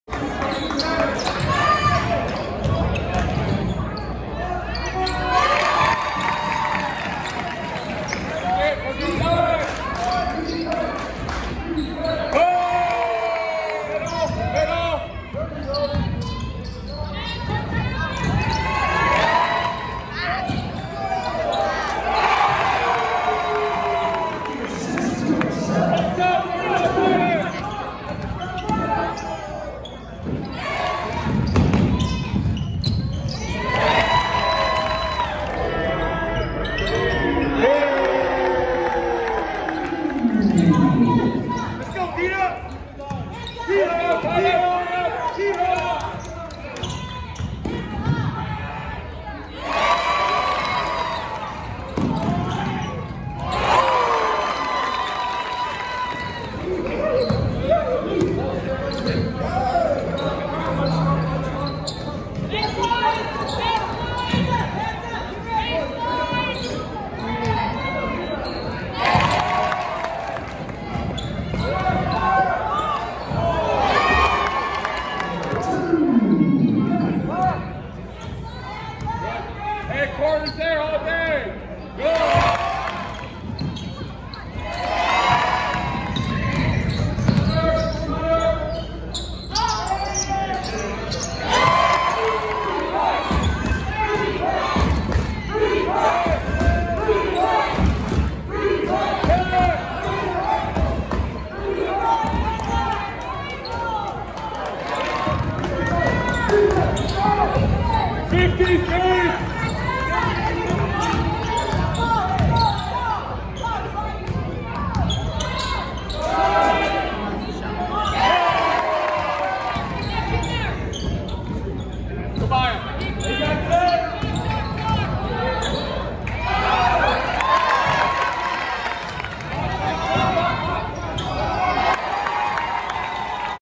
1Lapwai vs North Central 2A WA
Small school Vs big school challenge

Lapwai with a big victory over strong Washington team from Spokane WA.